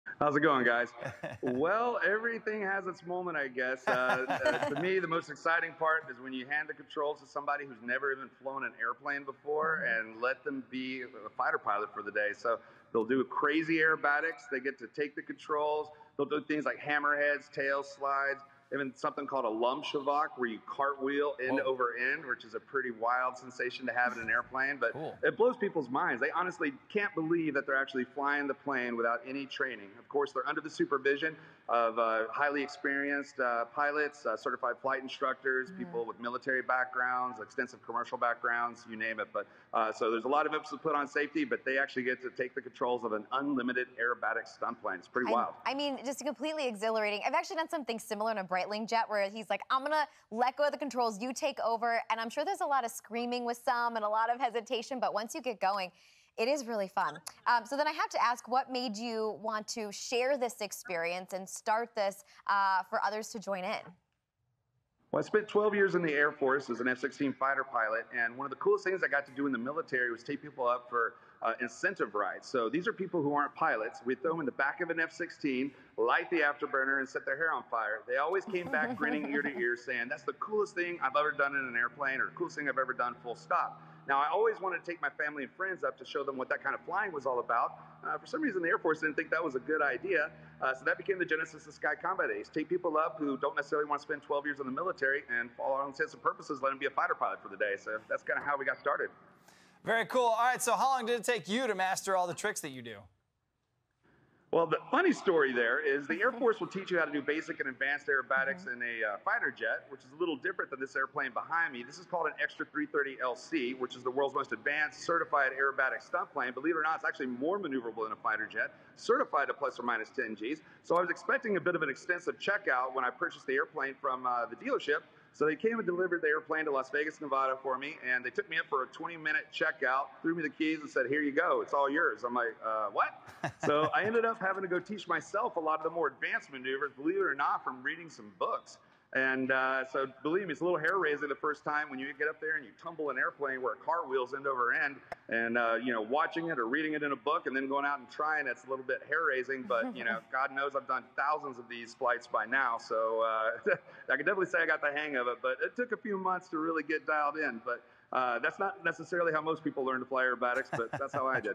Air Force Veteran Offers Thrill Seekers A Chance To Fly Stunt Plane
Sky Combat Ace CEO Richard ‘Tex’ Coe on allowing people to take the controls of an aerobatic stunt plane under the supervision of well-trained flight instructors.